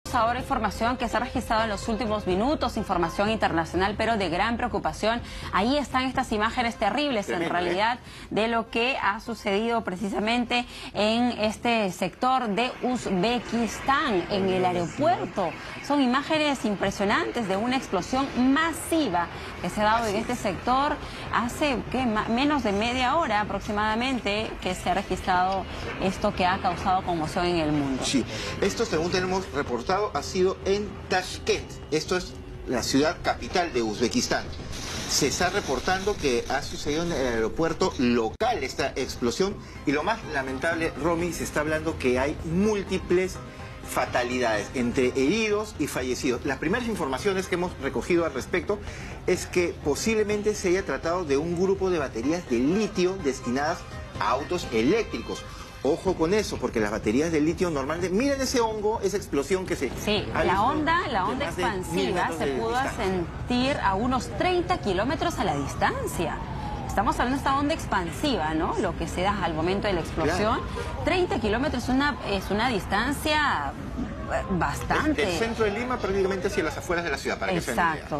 Fuerte explosión cerca del aeropuerto de la capital de Uzbekistán
Una potente explosión tuvo lugar en la madrugada del jueves cerca del aeropuerto de Tashkent, la capital de Uzbekistán, dijeron los medios de este país de Asia Central, cuyas autoridades informaron de un "incendio" que causó heridos.